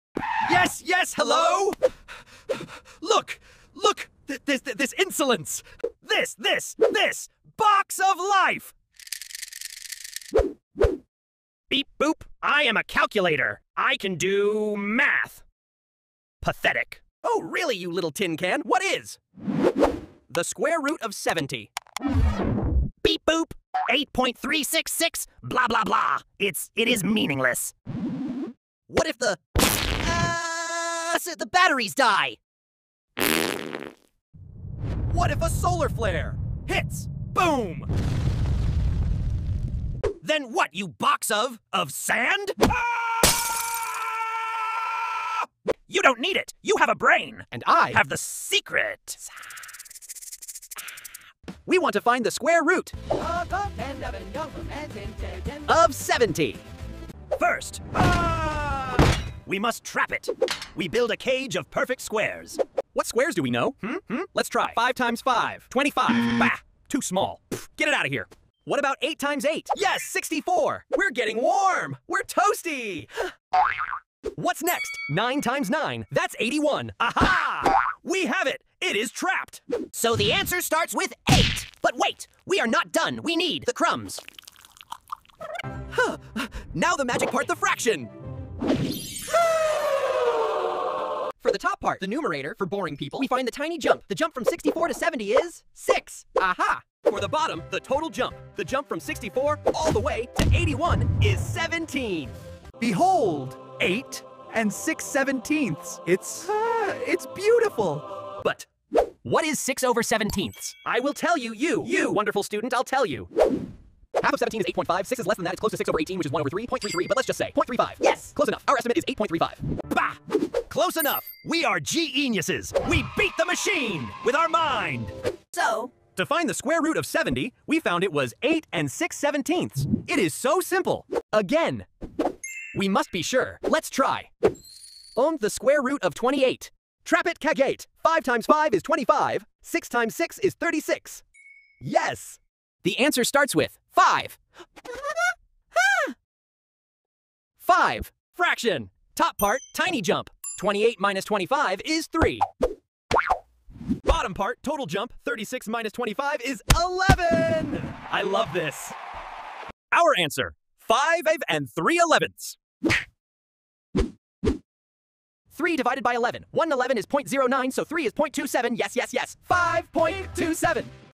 Estimate Square Roots in Your Head! (No Calculator Trick)
Unlock the secrets to solving math problems fast! In this video, I’ll show you a simple trick that makes even tough questions easy. Perfect for students, exam prep, and anyone who loves brain challenges. Watch now and boost your math IQ in minutes!